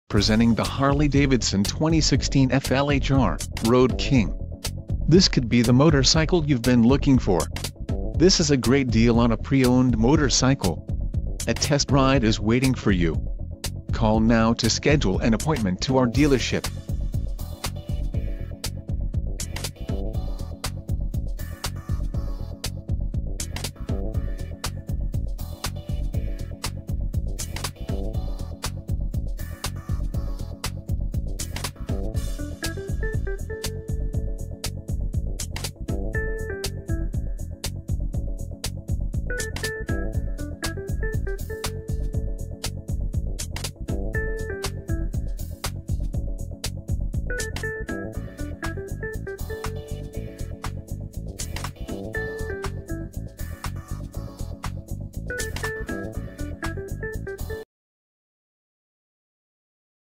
2016 Harley-Davidson® FLHR - Road King®
2016 Harley-Davidson and #174; Road King and #174;With this timeless combination of style and touring function you don and #8217;t just ride through the landscape, you and #8217;re the most majestic part of the scenery.
Even your eyes can and #8217;t stop wandering on this machine.

Look at the big windshield and nostalgic headlamp and you see a strong resemblance to the original King of the Highway. You could get lost for a good, long time just looking at the classic styling details. What you might not notice is how far it and #8217;s all come. You now have 103 cubic inches of High Output power. Dual halogen headlights. One-touch saddlebags. Reflex and #8482; linked brakes with ABS. And plenty more where that came from. Let yourself go.
Features may include:
Control